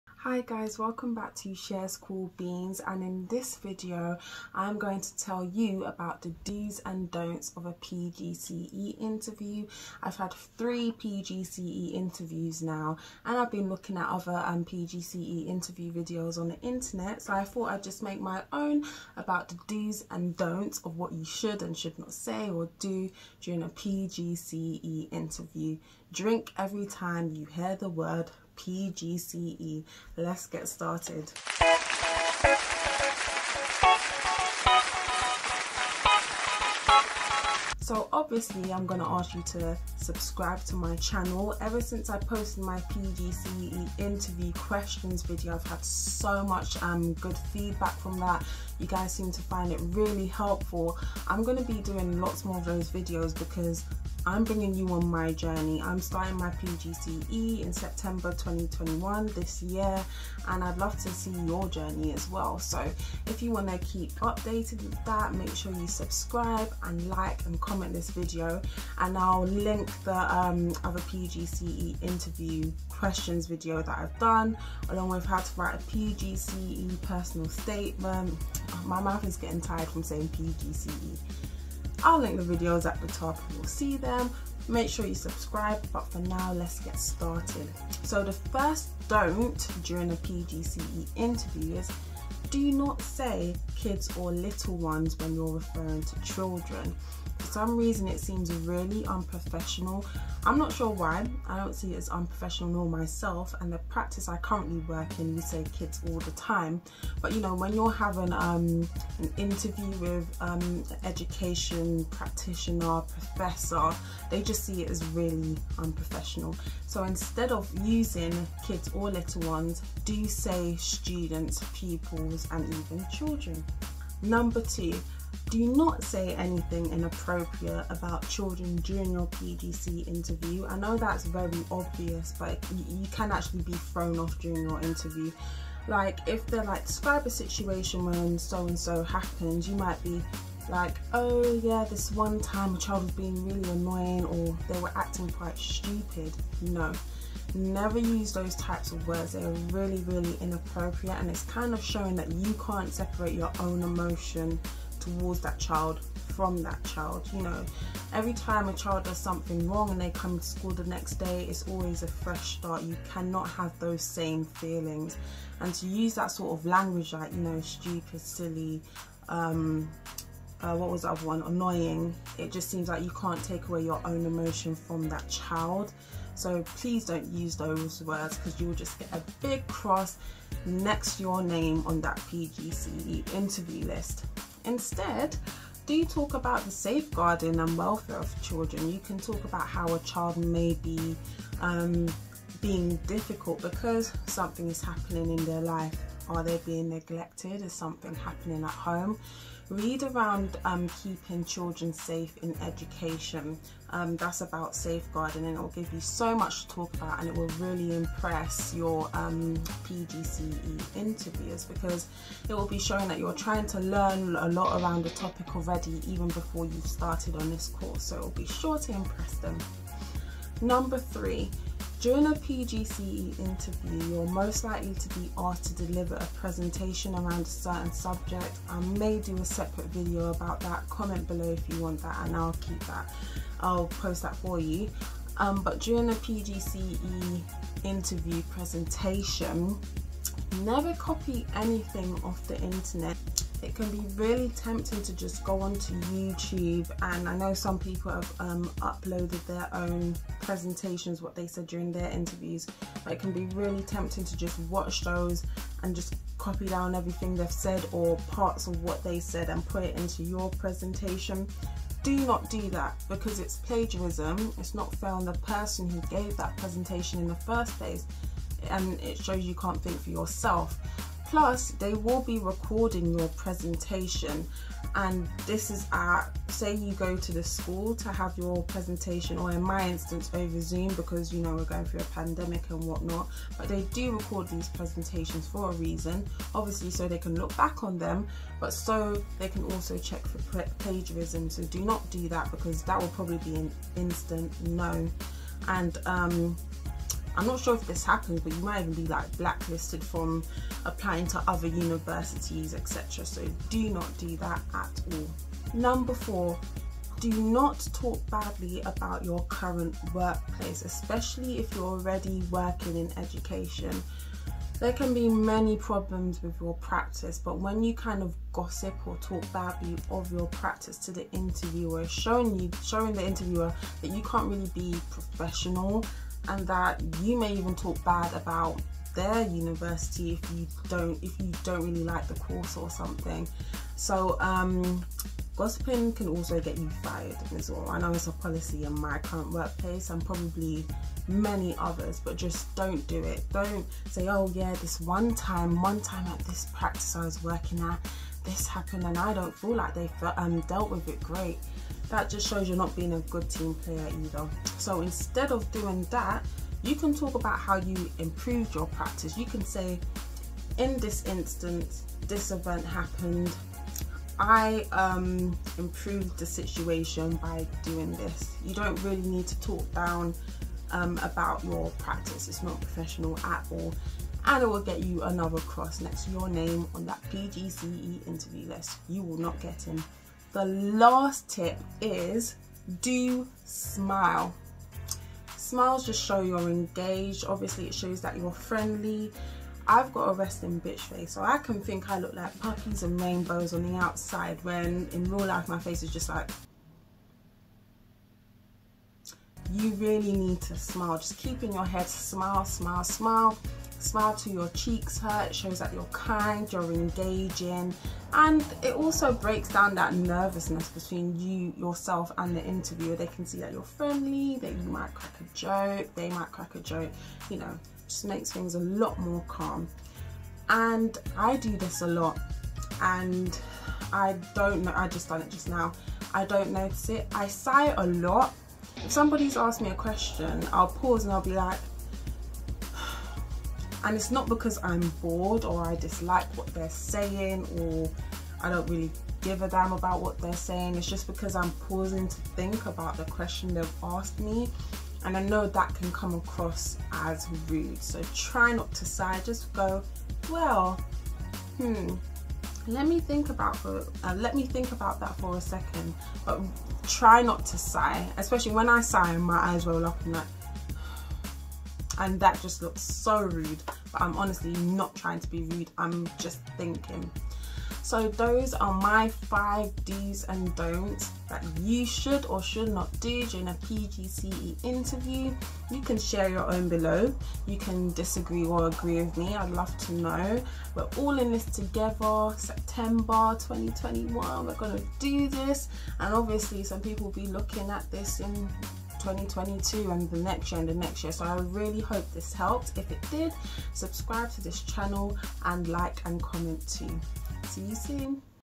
PGCE INTERVIEW: PGCE INTERVIEW DOs & DON'Ts!!! 📚🍎
Hi guys 💫

In this video I'll be talking about the Dos and Don'ts of a PGCE INTERVIEW!